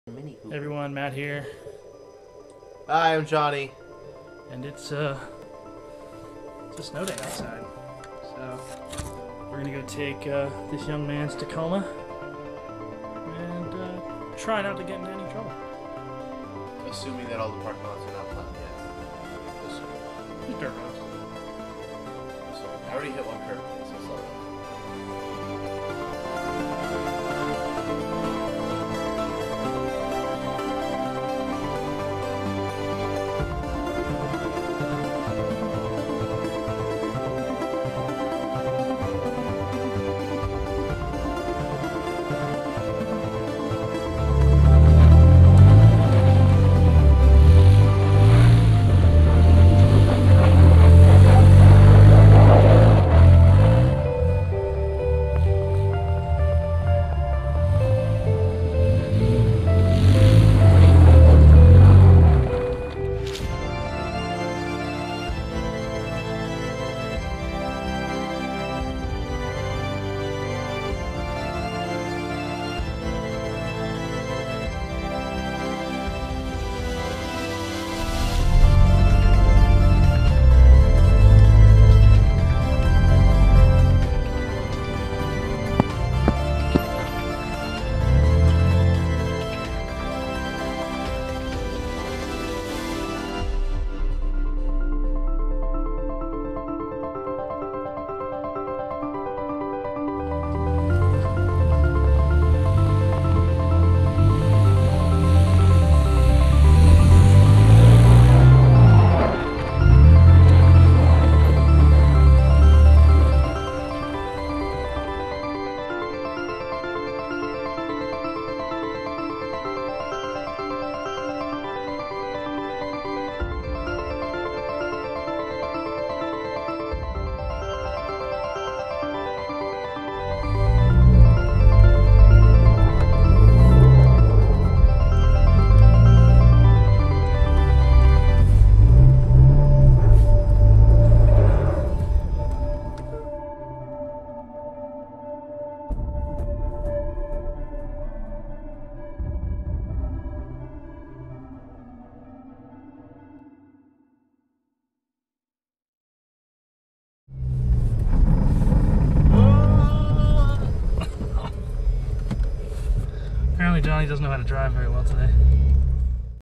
Tacoma Snow Drift : SO Motorsports
We take the SO Motorsports Tacoma out for a little fun in the snow!